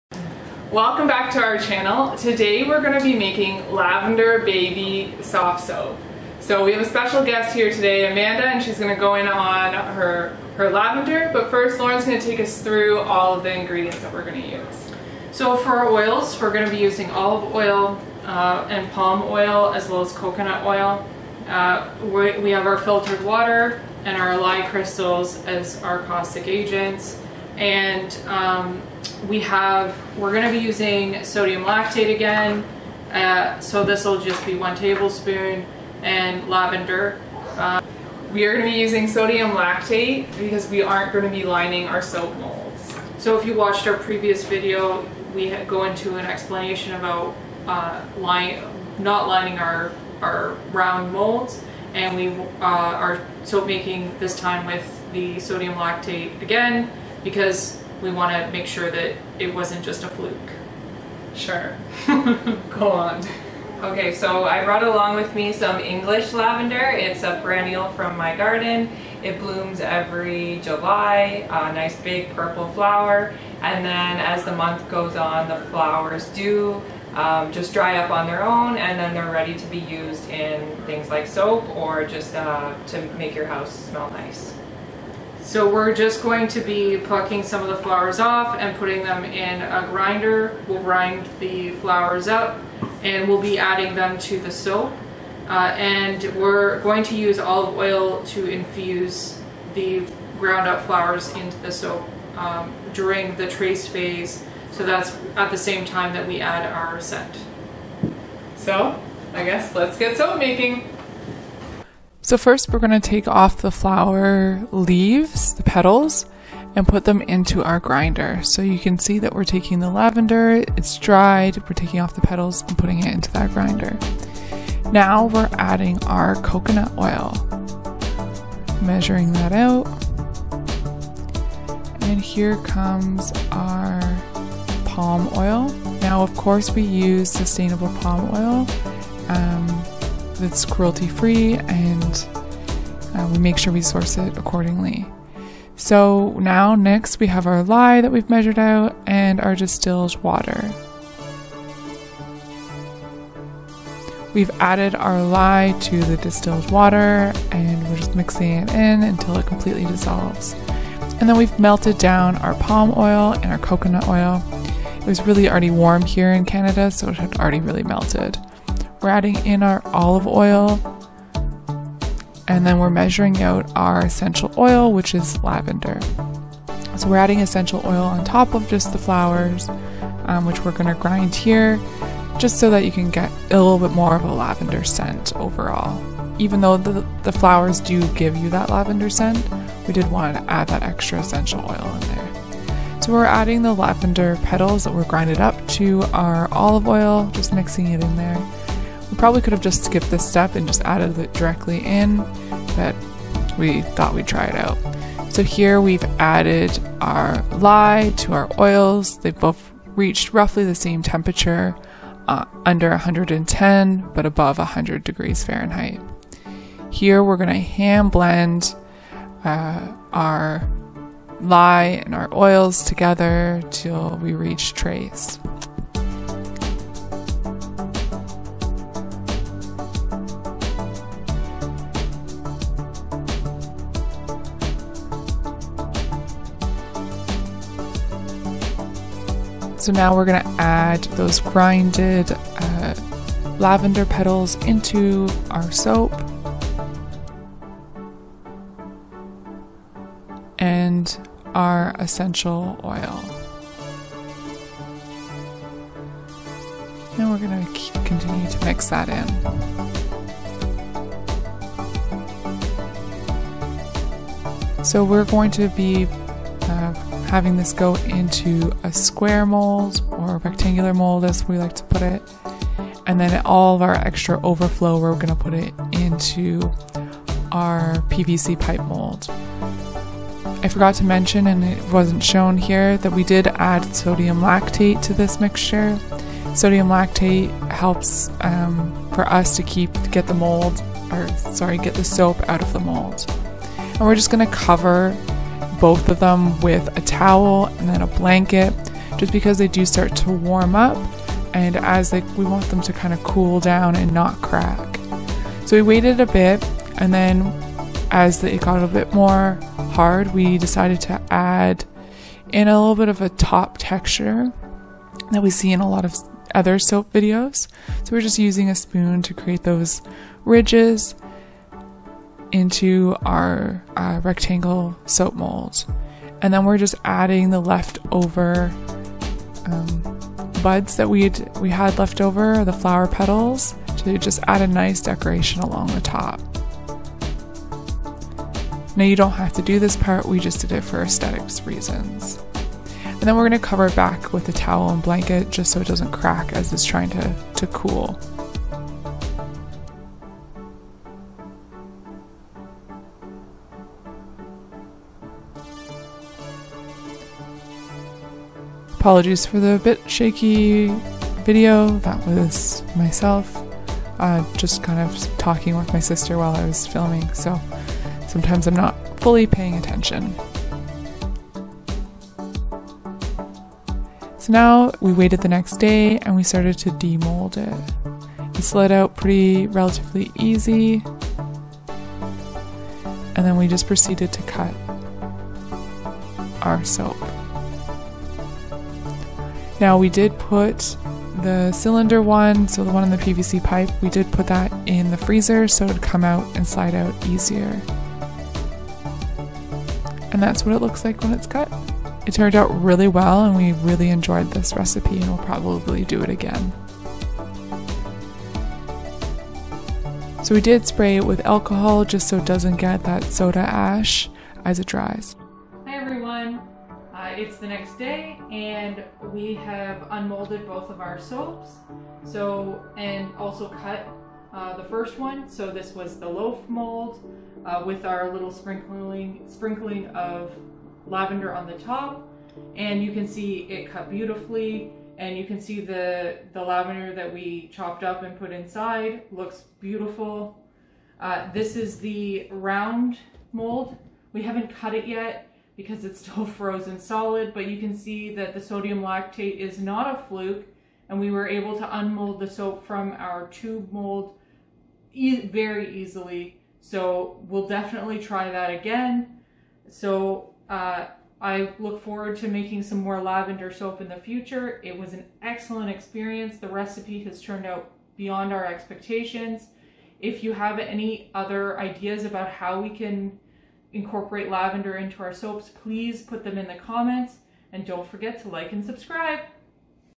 Soap Making: Adding dried lavender to your soap
Using dried lavender from our sister's garden, we infused it with our soap to make Lavender Baby Soft Soap.

Please be kind in the comments below.